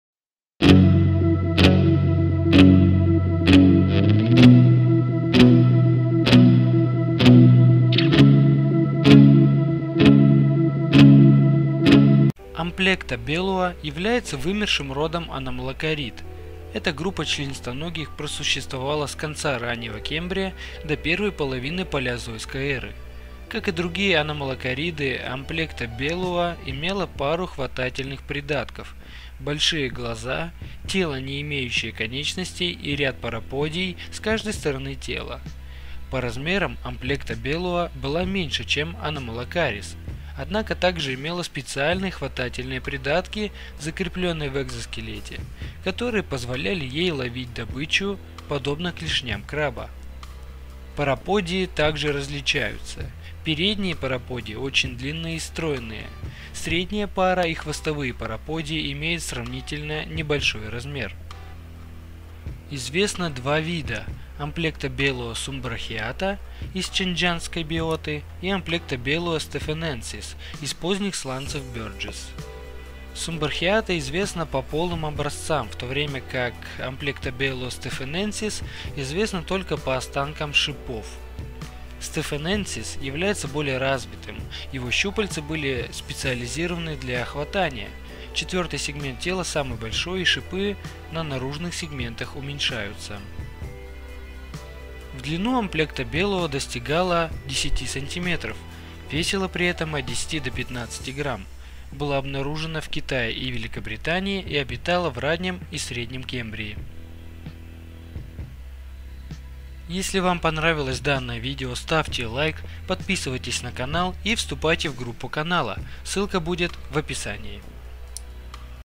Развитие жизни на Земле (The Evolution of Life) - Амплектобелуа (Amplectobelya)
Амплектобелуа является вымершим родом аномалокарид, эта группа членистоногих просуществовала с конца раннего кембрия до первой половины палеозойской эры. Как и другие аномалокариды, Амплектобелуа имела пару хватательных придатков, большие глаза, тело не имеющее конечностей и ряд параподий с каждой стороны тела. По размерам Амплектобелуа была меньше, чем Аномалокарис, однако, так же имела специальные хватательные придатки, закреплённые в экзоскелете, которые позволяли ей ловить добычу, подобно клешням краба. Параподии также различаются; передние параподии очень длинные и стройные, а средняя пара и хвостовые параподии имеют сравнительно небольшой размер.